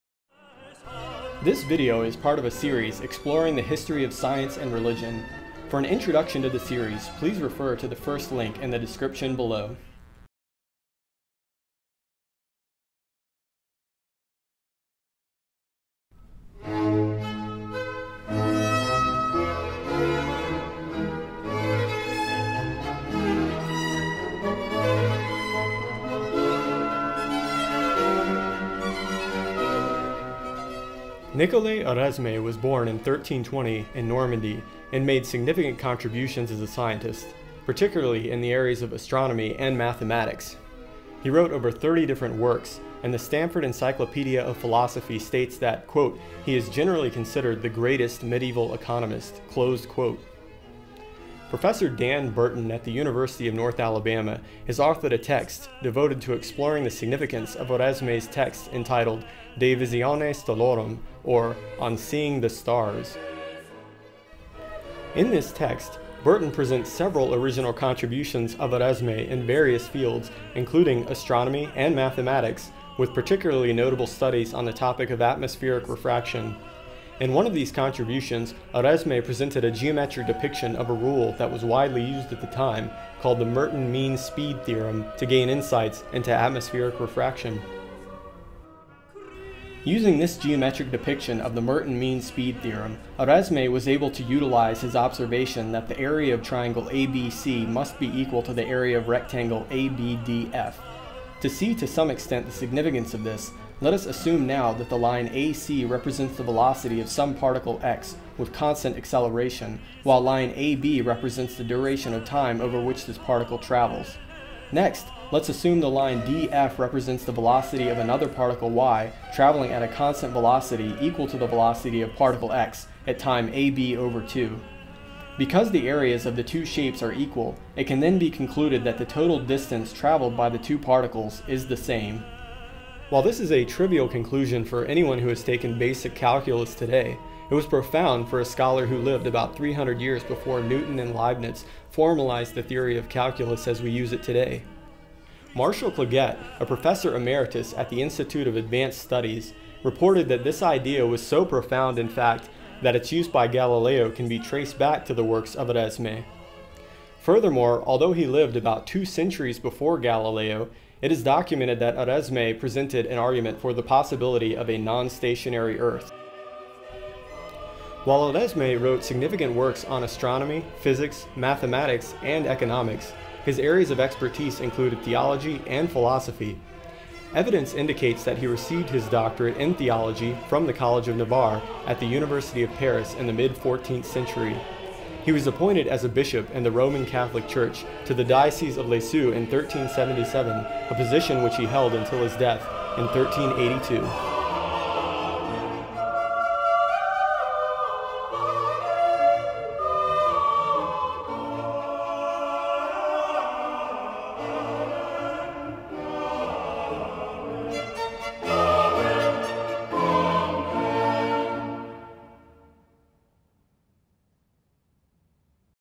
Liberation from Ignorance: Science vs. Religion, Nicole Oresme
For an introduction to this series on the history of science and religion, please see this video

It is a widely held notion that religion and science are inherently in conflict with one another. Can history offer insights into this question? We inspect some pertinent historical evidence by considering the life work of Nicole Oresme, a French scientist who lived in the 14th century.

References:
Burton, D., 2007, "Nicole Oresme's De visione stellarum (On Seeing the Stars). A Critical Edition of Oresme's Treatise on Optics and Atmospheric Refraction, with an Introduction, Commentary, and English Translation." Leiden, Boston: Brill.

Clagett, M., 1974, "Oresme, Nicole," in Dictionary of Scientific Biography, Vol. X, Ch. C. Gillispie (ed.), New York: Charles Scribner's Sons.

Kirschner, Stefan, "Nicole Oresme", The Stanford Encyclopedia of Philosophy (Fall 2009 Edition), Edward N. Zalta (ed.), URL =